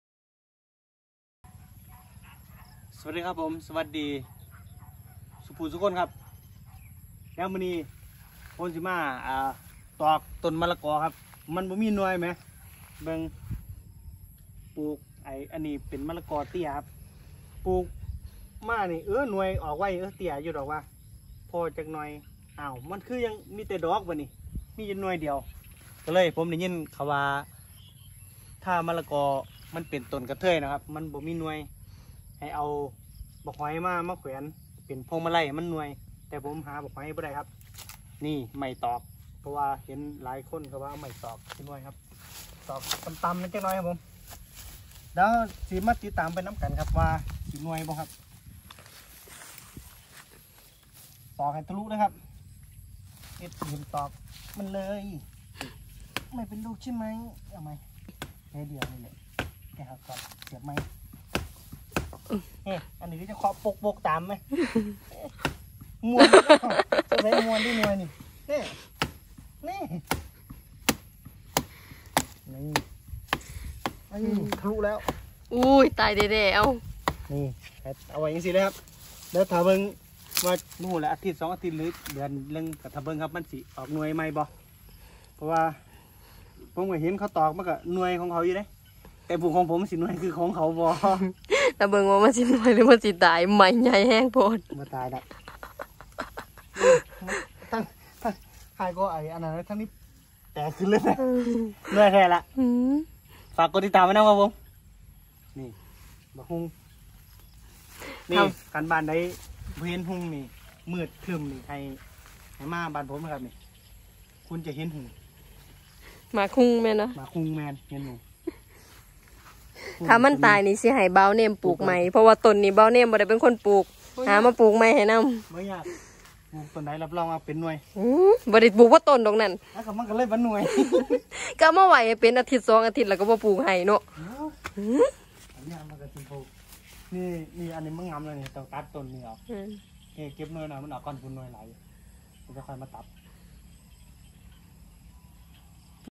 การทำให้ต้นมะละกอติดลูกวิธีชาวบ้าน@BawName_surapon
นำไม้ไผ่มาเหลาให้เหงียมแล้วก็เอาไปเสียบตอกตรงโคนต้นมะละกอให้ทะลุ